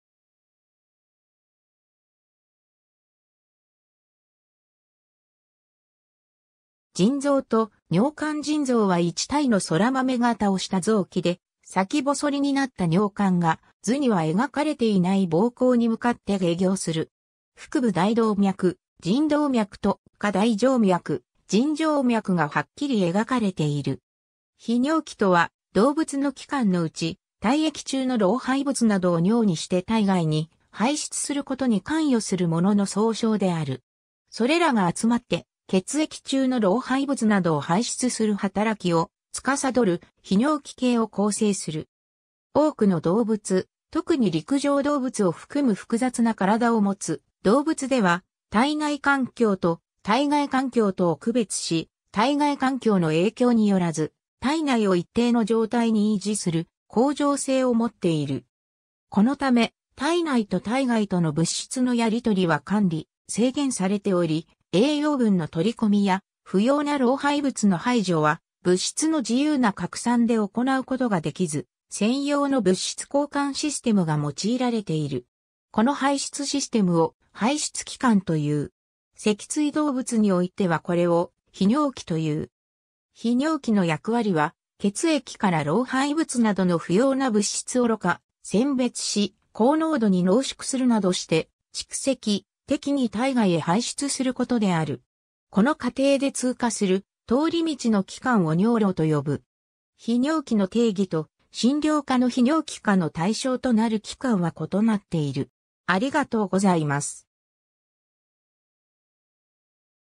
泌尿器
腎臓と尿管 腎臓は1対のソラマメ型をした臓器で、先細りになった尿管が図には描かれていない膀胱に向かって下行する。
腹部大動脈、腎動脈（赤）と下大静脈、腎静脈（青）がはっきり描かれている。
 泌尿器（ひにょうき、英語: urinary organs）とは、動物の器官（臓器）のうち、体液（血液）中の老廃物などを尿にして体外に排出することに関与するものの総称である。
それらが集まって血液中の老廃物などを排出する働きを司る泌尿器系（ひにょうきけい、英語: urinary system）を構成する。
 多くの動物、特に陸上動物を含む複雑な体を持つ動物では、体内環境と体外環境とを区別し、体外環境の影響によらず、体内を一定の状態に維持する恒常性を持っている。
このため、体内と体外との物質のやり取りは管理・制限されており、栄養分の取り込みや、不要な老廃物の排除は、物質の自由な拡散で行うことができず、専用の物質交換システムが用いられている。
この排出システムを排出器官と言う。
脊椎動物においてはこれを泌尿器と言う。
 泌尿器の役割は、血液から老廃物などの不要な物質を濾過・選別し、高濃度に濃縮するなどして蓄積、適宜体外へ排出することである。
この過程で通過する通り道の器官を尿路と呼ぶ。
 泌尿器の定義と、診療科の泌尿器科の対象となる器官は異なっている。